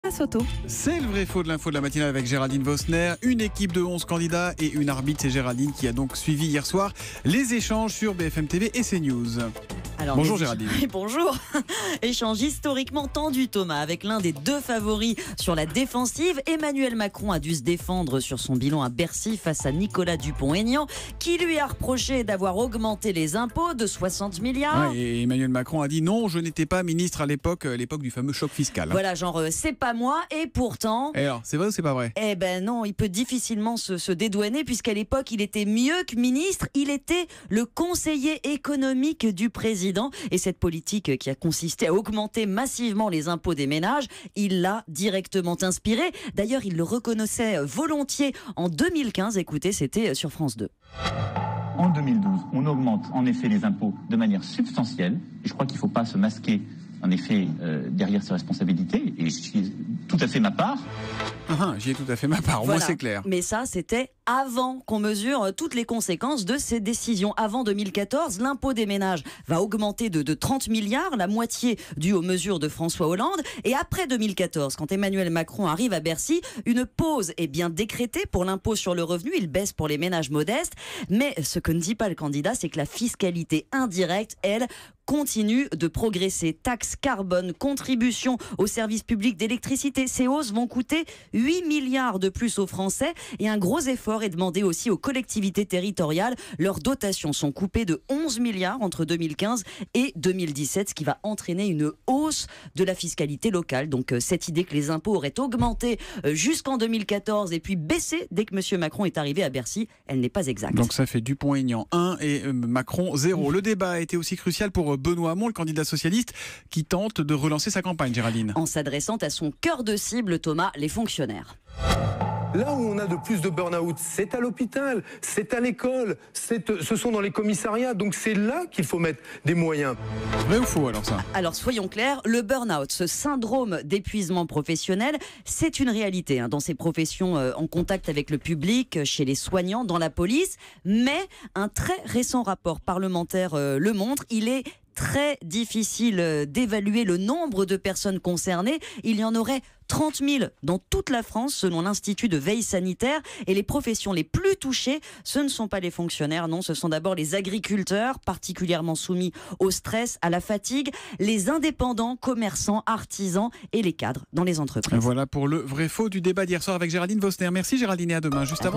Débat : Emmanuel Macron sur la défensive
Le favori de la campagne a inspiré l'augmentation massive des ménages sous François Hollande.